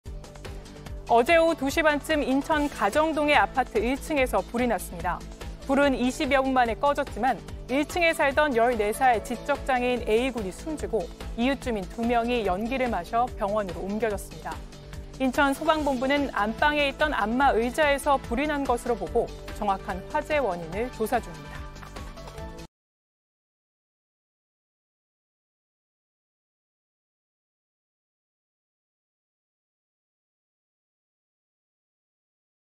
인천 아파트서 불…지적장애 10대 사망, 2명 연기 흡입 / JTBC News
어제(6일) 오후 2시 반 쯤 인천 가정동의 아파트 1층에서 불이 났습니다.

불은 20여 분만에 꺼졌지만, 1층에 살던 14살 지적장애인 A군이 숨지고 이웃 주민 2명이 연기를 마셔 병원으로 옮겨졌습니다.

인천소방본부는 안방에 있던 안마 의자에서 불이 난 것으로 보고 정확한 화재 원인을 조사 중입니다.